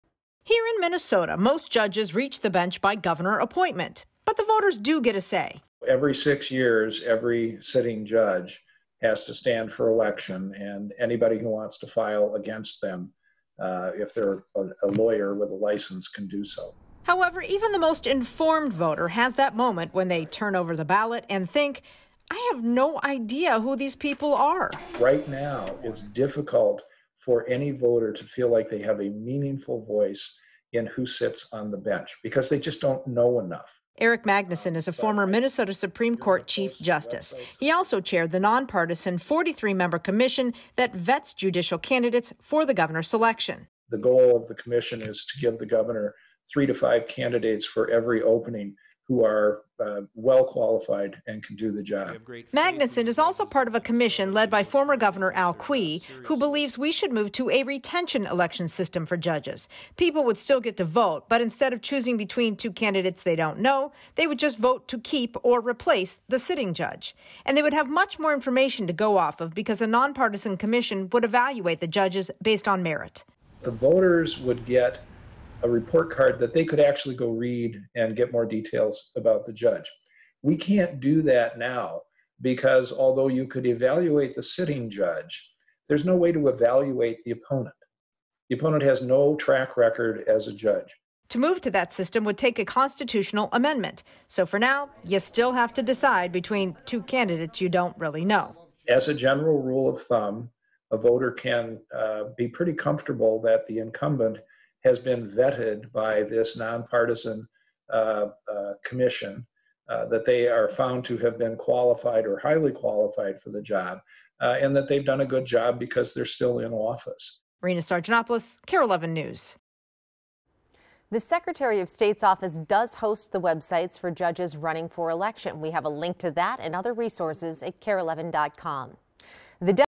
Voting for judges: Why it's important and why some think the way we do it should change
Rena tells us why voting for judges is important.